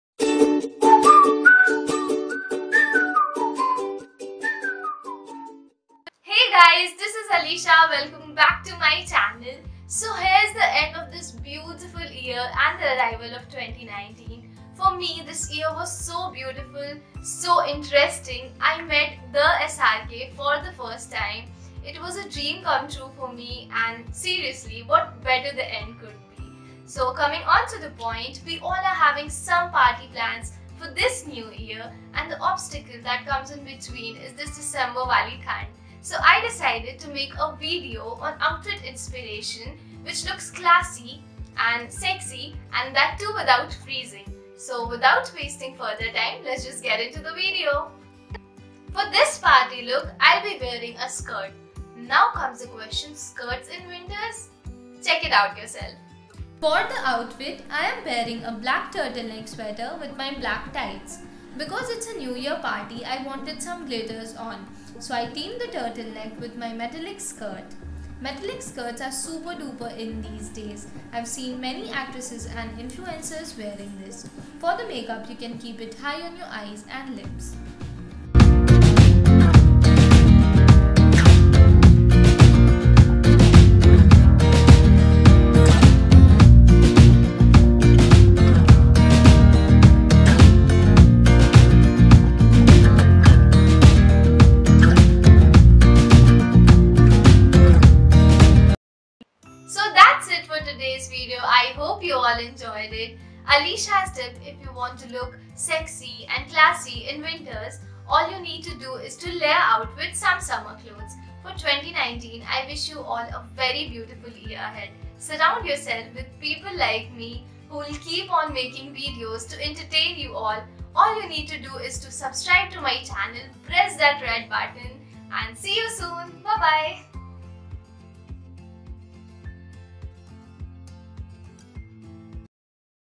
New Year Party | Be the Sexiest | Skirts in winters?? | Alisha Singh
So I decided to make a video on how to look sexy and classy for a new year party and that too without freezing.
I will be giving you some idea about the perfect new year party look.

Give the video a thumbs up if you liked the video.

Love
~Alisha

For bussiness enquiries and collaboration~